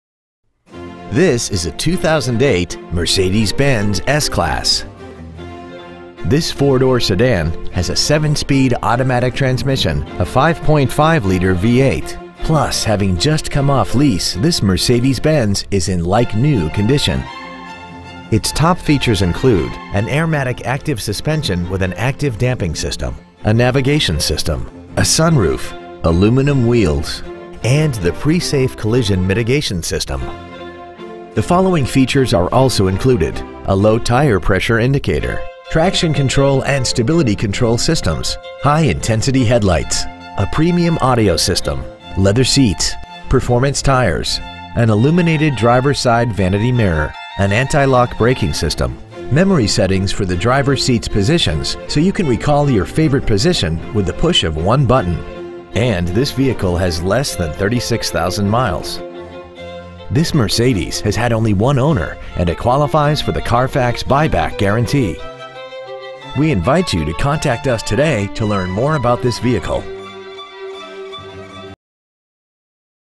Used 2008 Mercedes-Benz 5.5L V8 Miami FL
Year : 2008

 Make : Mercedes-Benz

 Model : 5.5L

 Engine : Gas V8 5.5L/333

 Trans . : Automatic

 Exterior : Black

 Miles : 35,803

 Interior : Cashmere/Savannah

 Stock : 35363

 Offleaseonly.com

 3531 Lake Worth Rd

 Used 2008 Mercedes-Benz 5.5L V8 Lake Worth FL